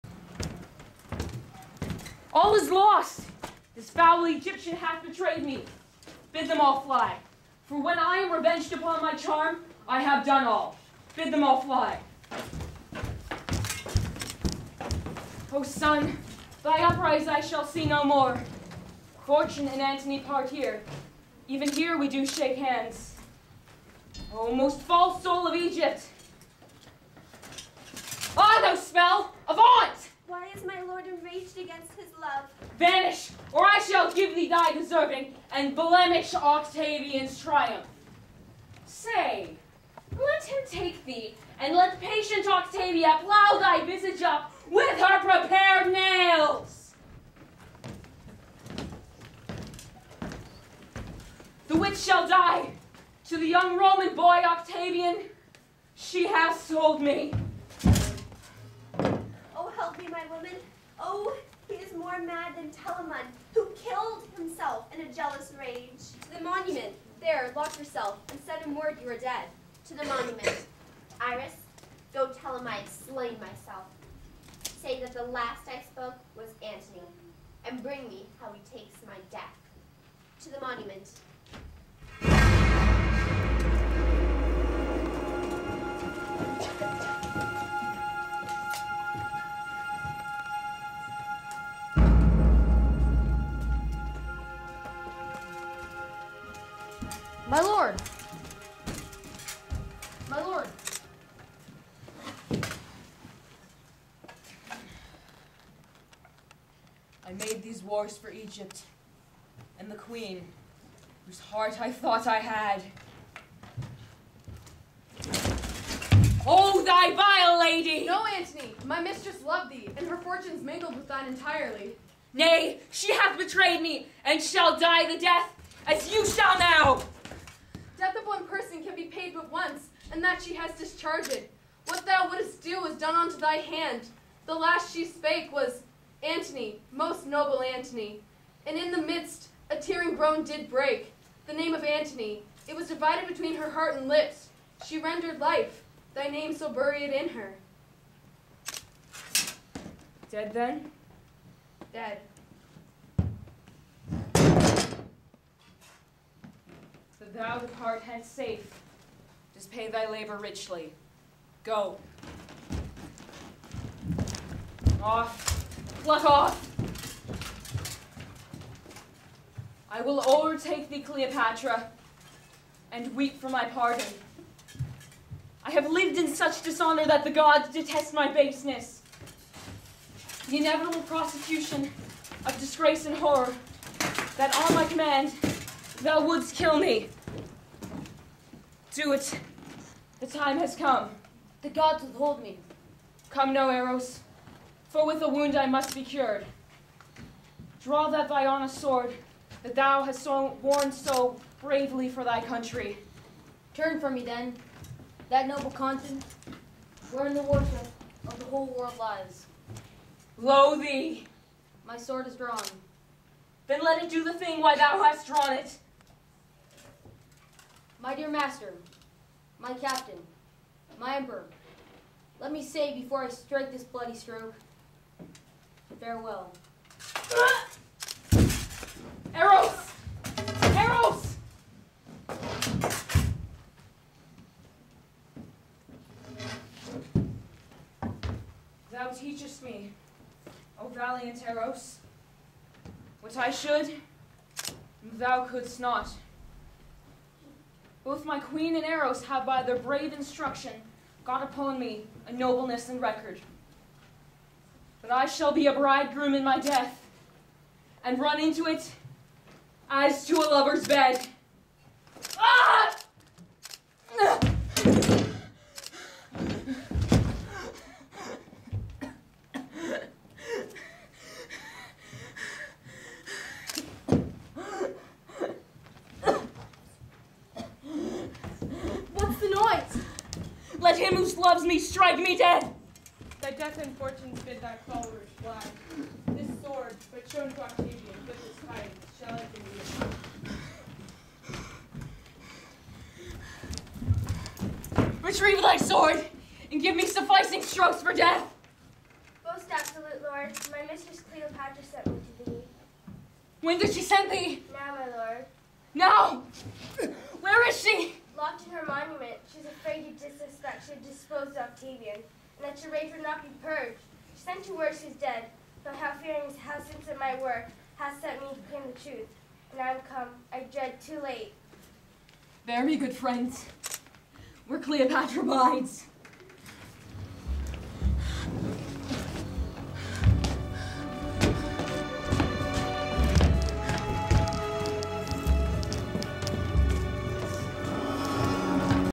William Shakespeare's Antony and Cleopatra (Part 3)
William Shakespeare's Antony and Cleopatra presented by the grade 8 students of Lester B Pearson school for the arts.